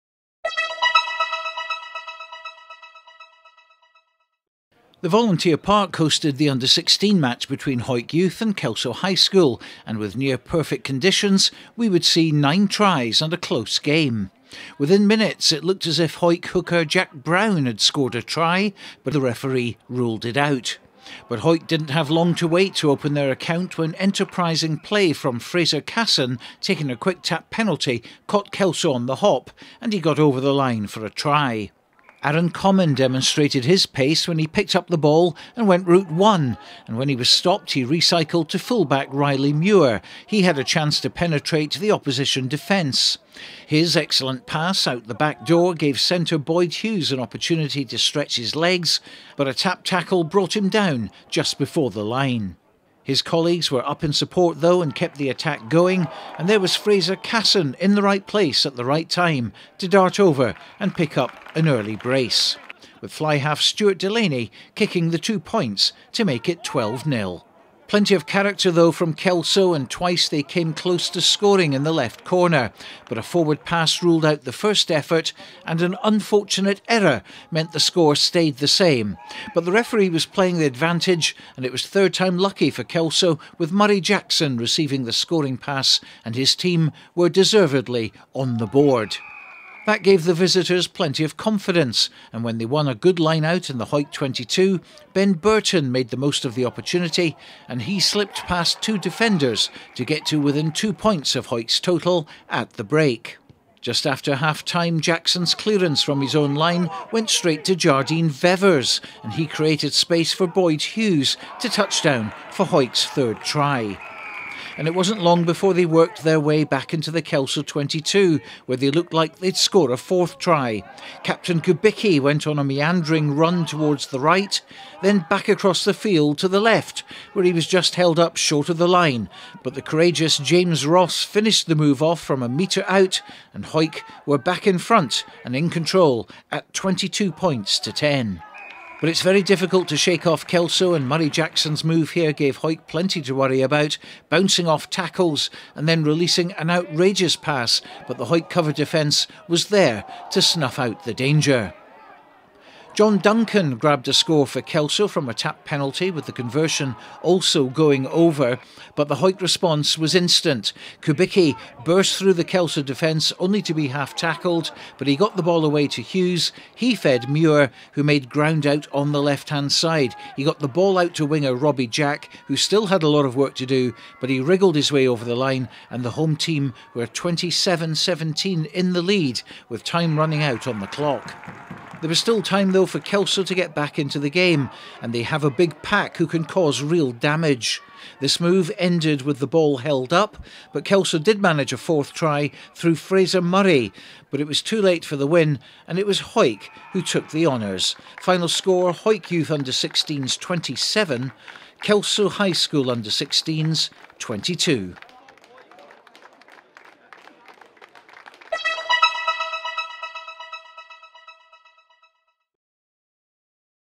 RUGBY REPORT: HAWICK YOUTH U16s v KELSO HS U16s - 14.1.22
Stuart Cameron reports from Volunteer Park, Hawick as Hawick Youth U16s team took on Kelso High School U16s.